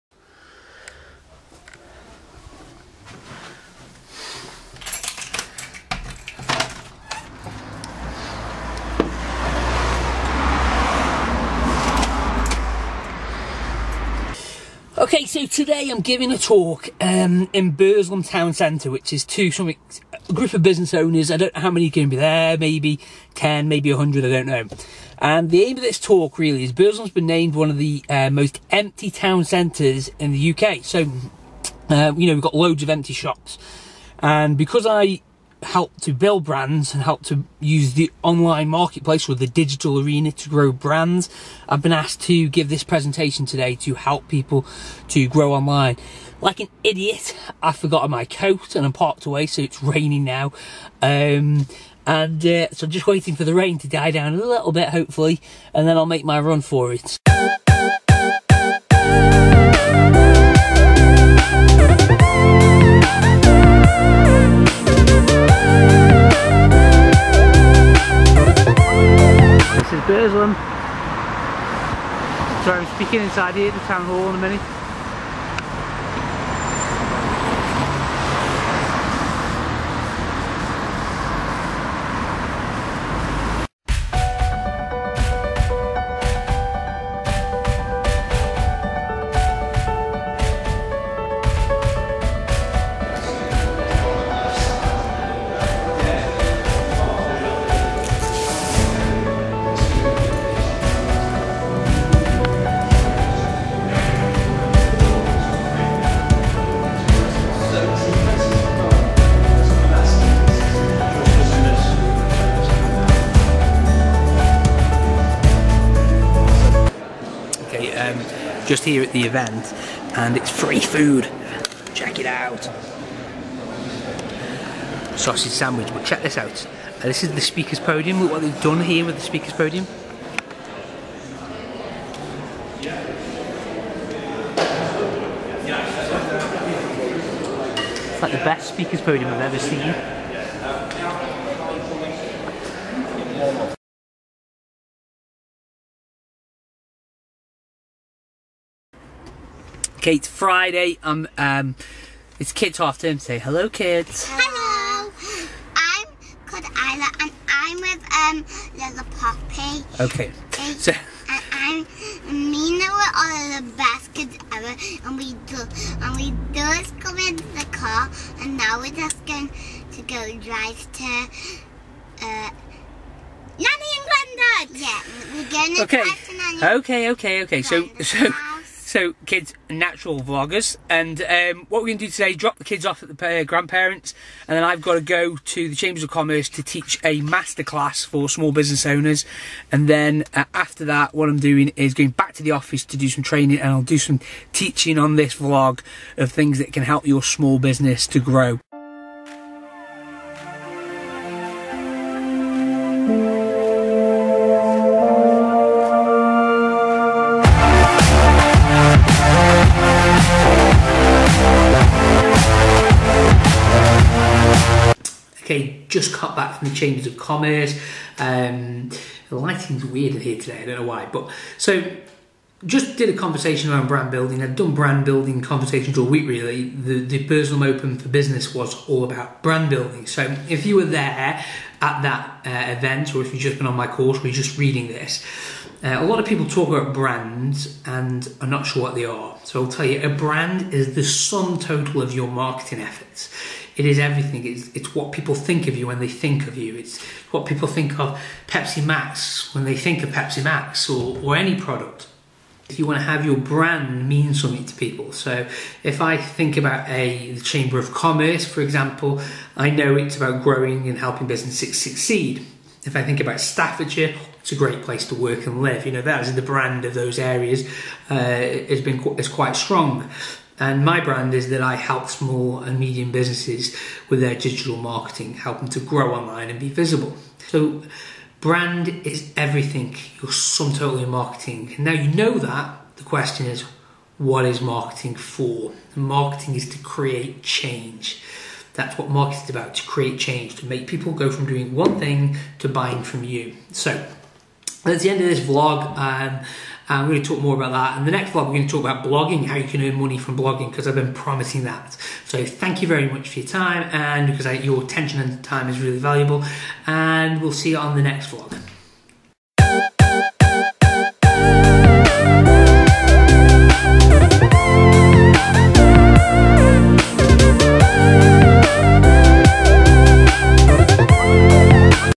Burslem is open for business Vlog Episode 5
Building a brand does not happen overnight and in the video I will share some of my insights on brand building and marketing.

If you want to learn more about who I am